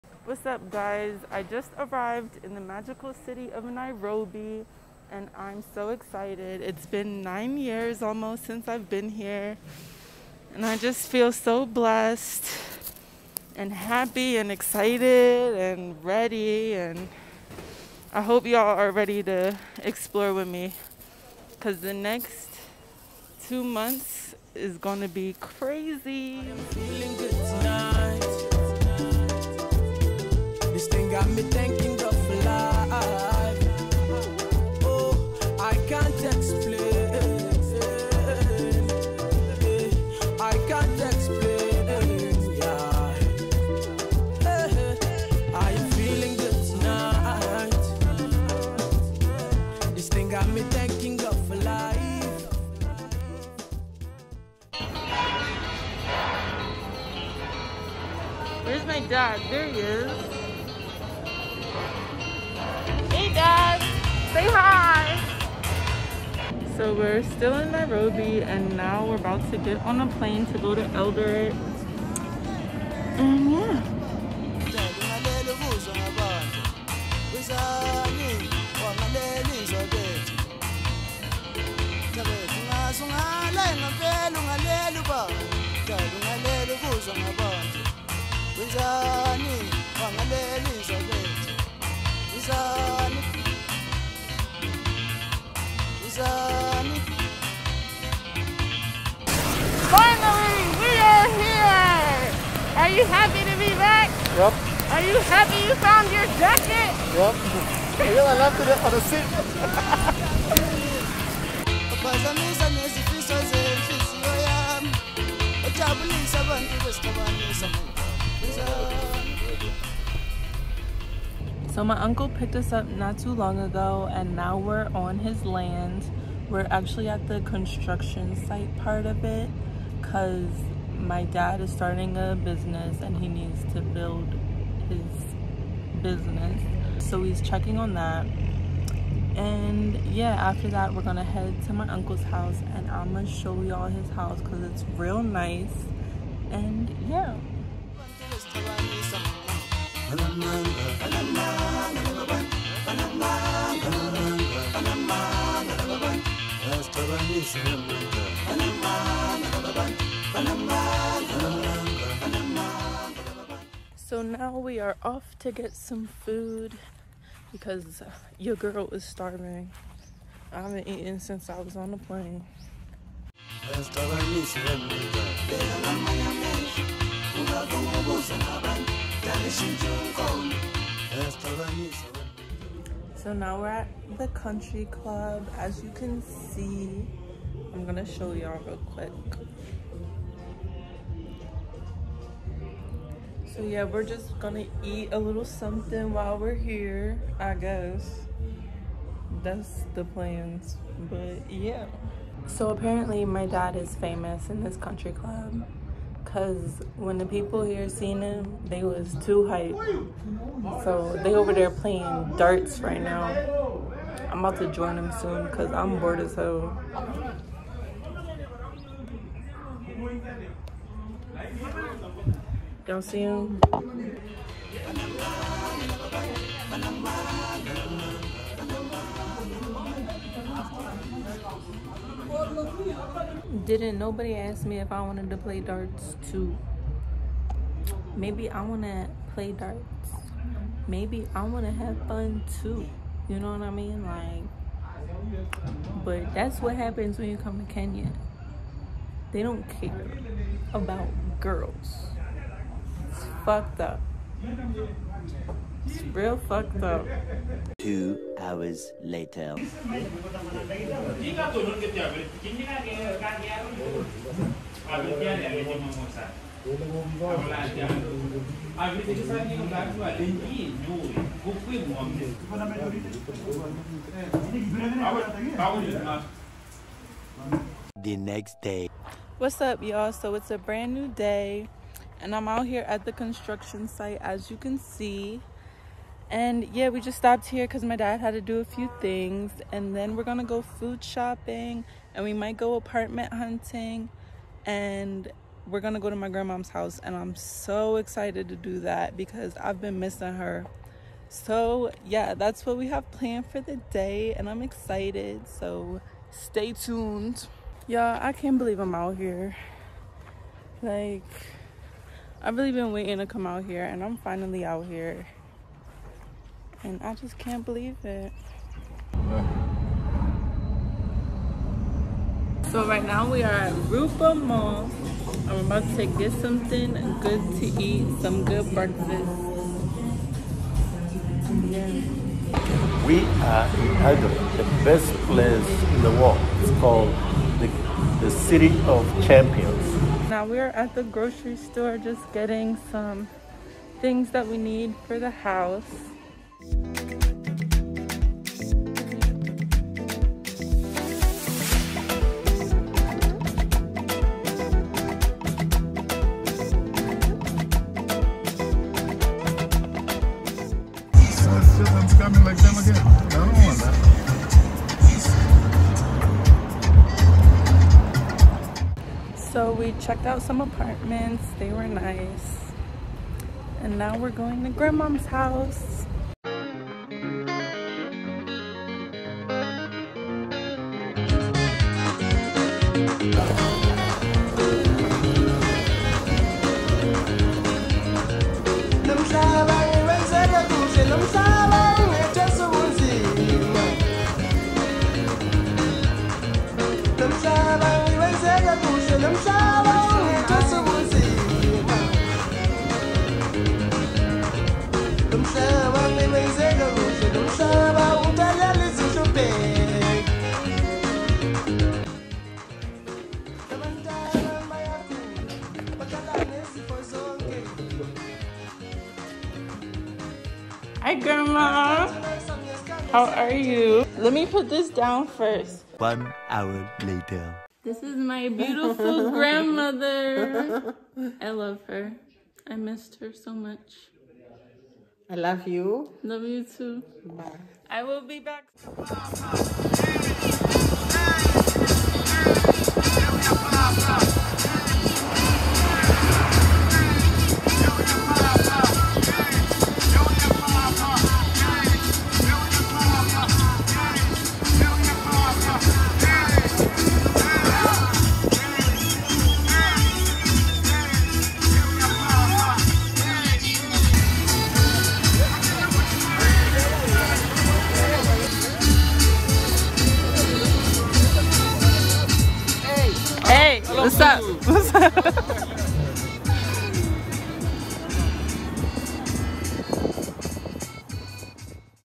FROM NEW YORK TO NAIROBI TO ELDORET | KENYA TRAVEL VLOG #1 | EXPLORING AFRICA | CIERRA MIST
Join me as I go from the U.S. to Kenya, aka the mother land.